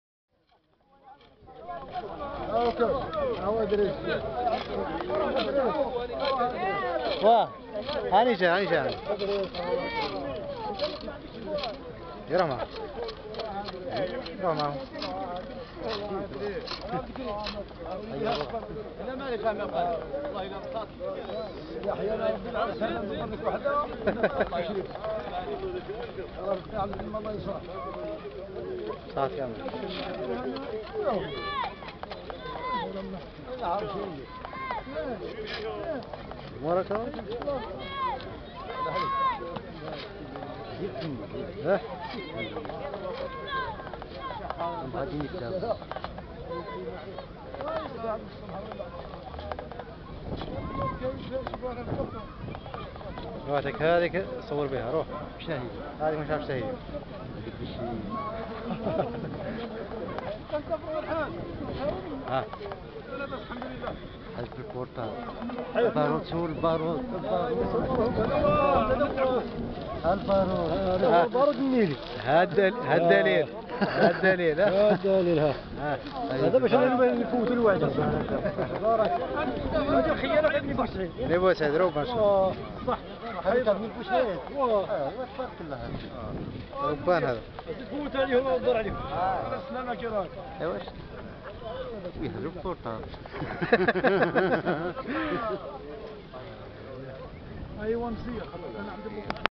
ouled moussa beni boussaid fete 1er novembre partie5
la fete du 1er novembre commencement de la guerre de la revolution algerienne quelle fete !!! du couscouss ,du leben ,du lait,  des dattes et des fruits a gogo , fantazia qui rappelle le courage de la chevalerie  de notre emir abdelkader ,l'allaoui  qui reflete la virilité  de l'algerien et sa  culture originale dans toute sa beauté ,la gasba et le bendir vous enchantent les  balancements harmoniques des pieds des epaules et du corps vous envoutent  .faites un tour chez nous l'année prochaine inchallah vous ne serez pas decus .